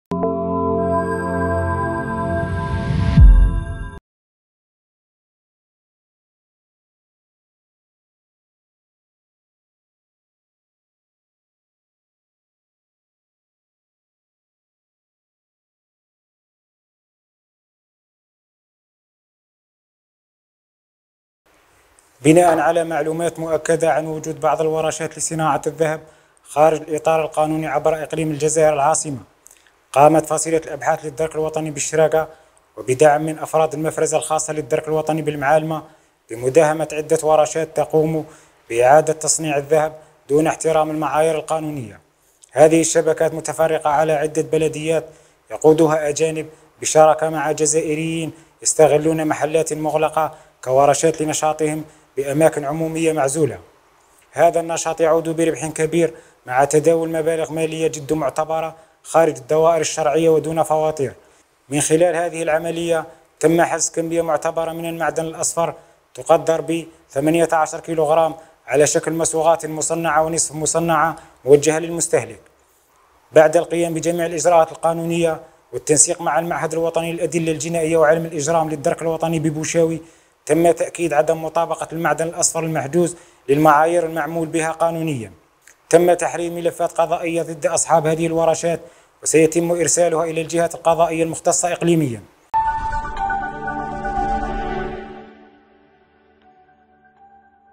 حجز 18 كلغ من الذهب المغشوش و وضع حد لورشات غير شرعية تنشط في صناعة المجوهرات
الجزائر(وأج)- تمكنت فصيلة الأبحاث للدرك الوطني بالشراقة، بدعم من أفراد المفرزة الخاصة لذات السلك، من تفكيك عدة ورشات تقوم بإعادة تصنيع الذهب دون احترام المعايير القانونية مع حجز 18 كلغ من المعدن المغشوش، حسب ما أفاد به بيان لمصالح الدرك الوطني.